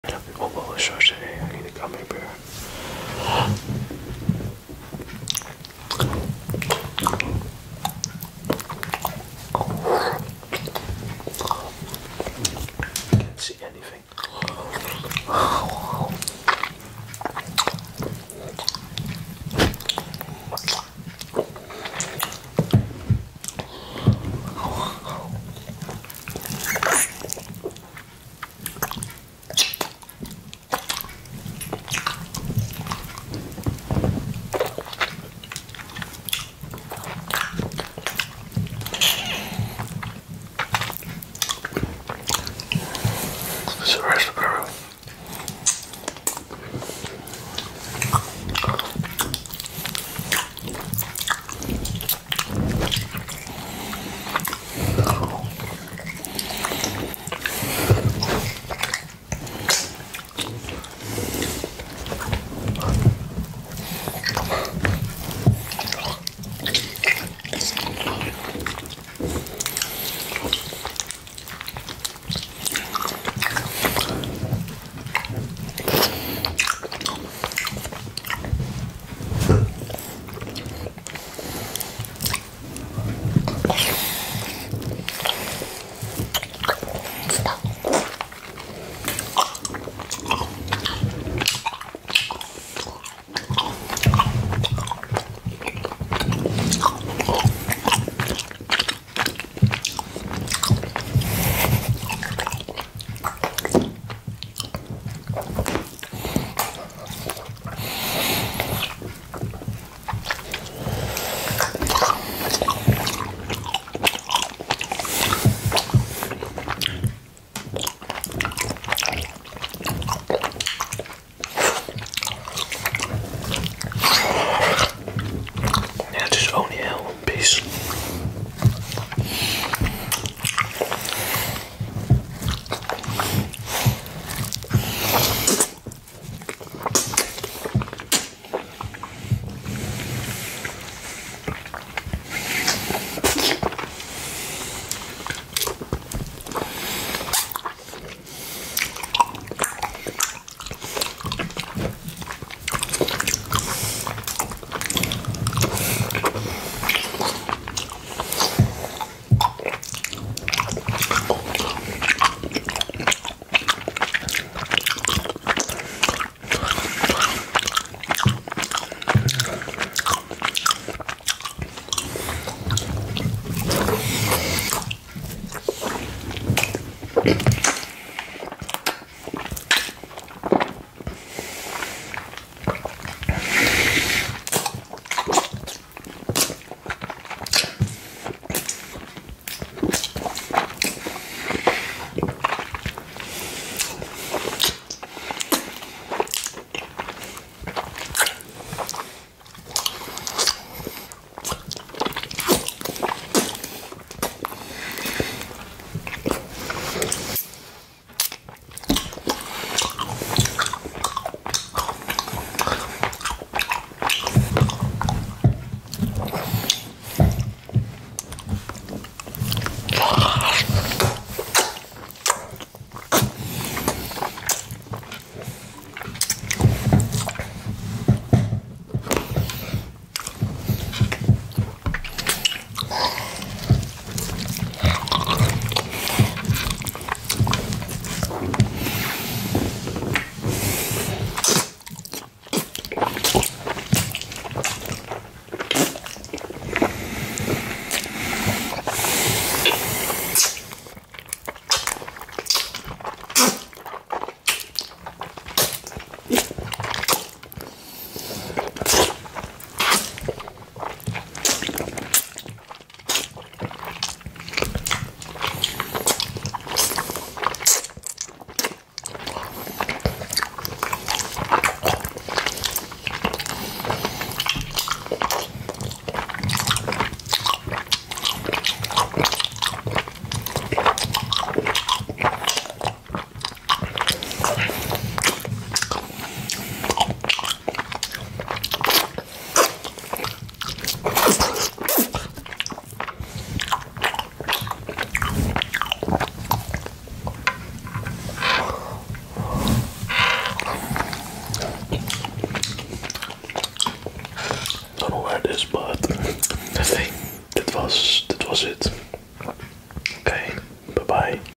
ASMR EATING Gummy Bear Blindfolded & Without Using Hands
In this ASMR video I'm eating gummy bear blindfolded and without using hands.